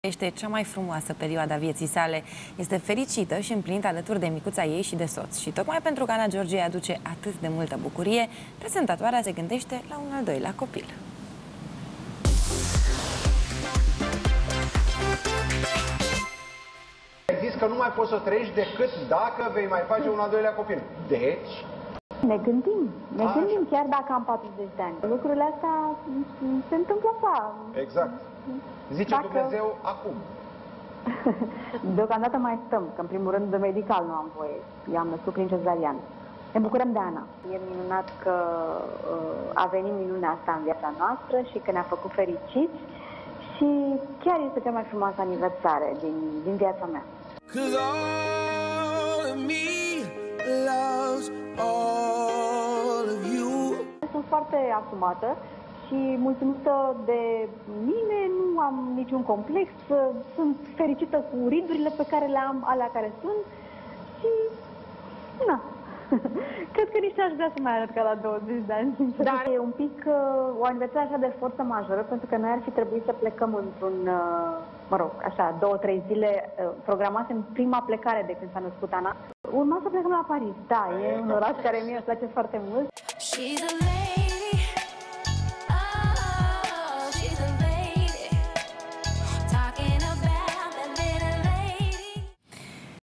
Simona Gherghe se gândește să devină din nou mamă
Simona Gherghe trăieşte cea mai frumoasă perioadă a vieții sale. Este fericită și împlinită alături de micuța ei și de soț! Și tocmai pentru că Ana Georgia îi aduce atât de multă bucurie, prezentatoarea tv se gândește la un al doilea copil